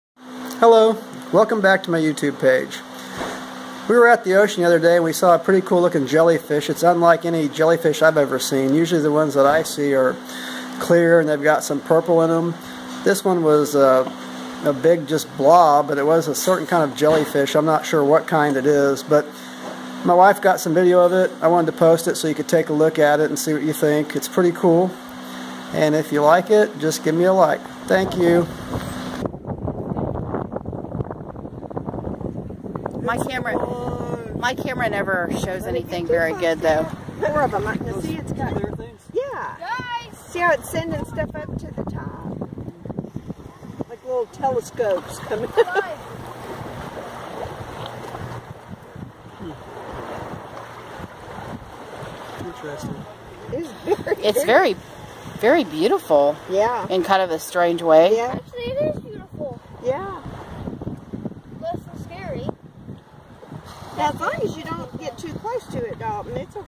Jellyfish at Pensacola Beach
Giant jellyfish at Pensacola Beach in shallow water.